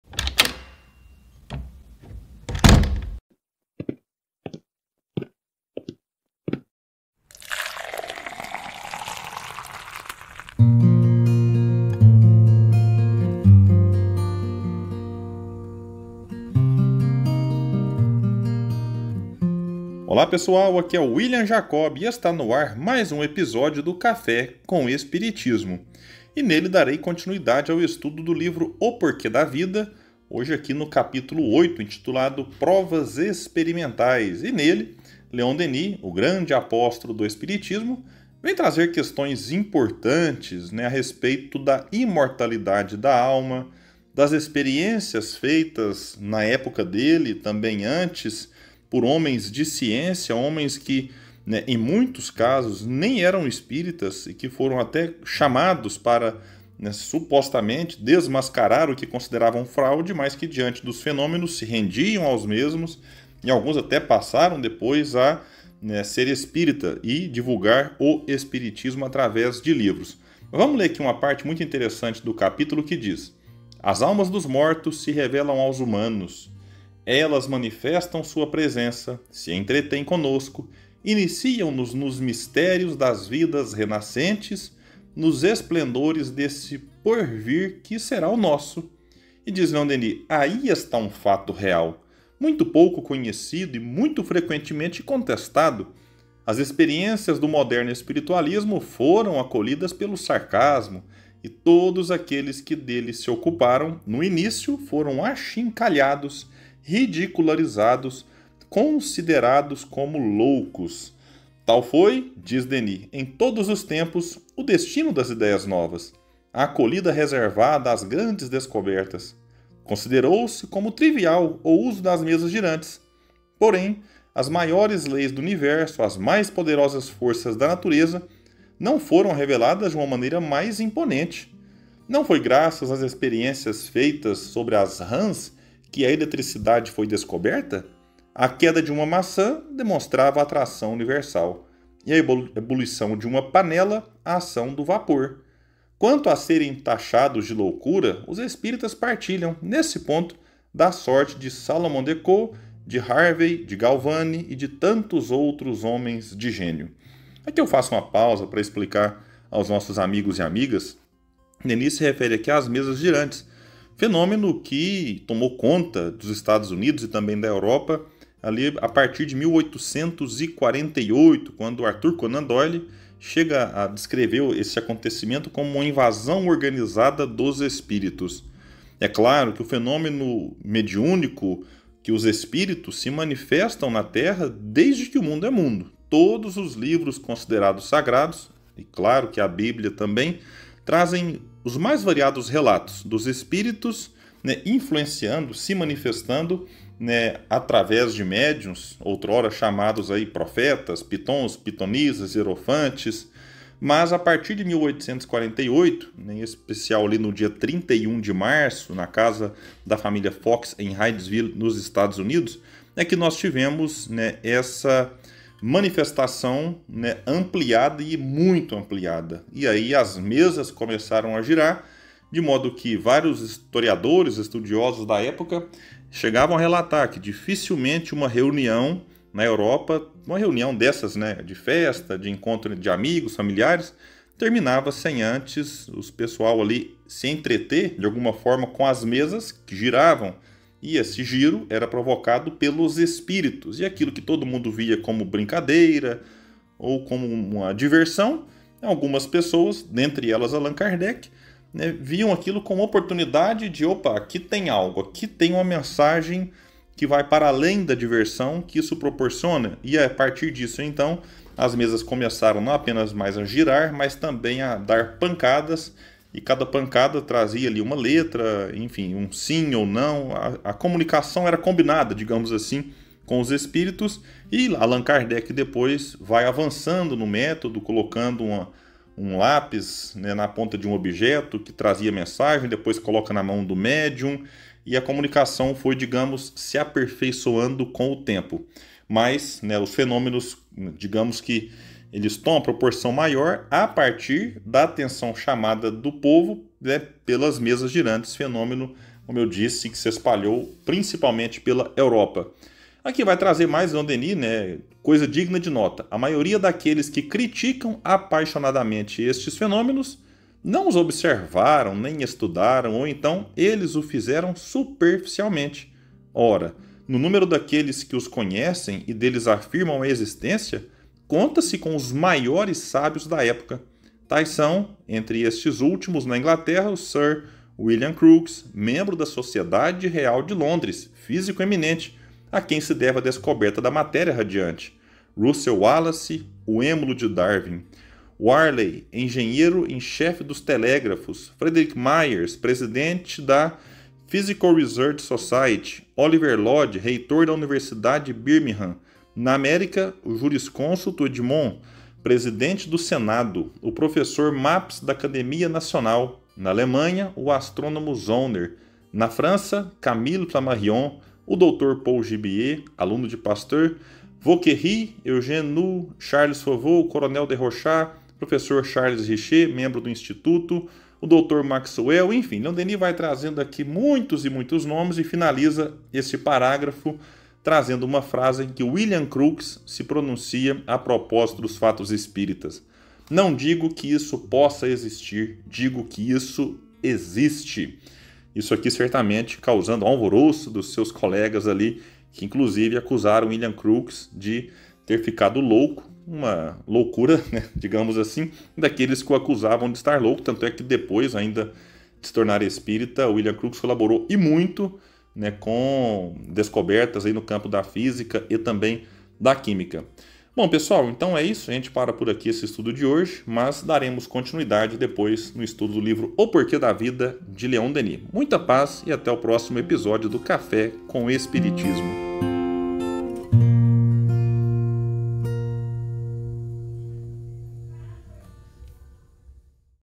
CaféComEspiritismo: 1560 - Provas Experimentais pt. 2 - William Jacob
Olá, pessoal!!

Mensagem de hoje: Provas experimentais - Parte 2
Livro: O Porquê da Vida
Autor: Léon Denis
Comentários: William Jacob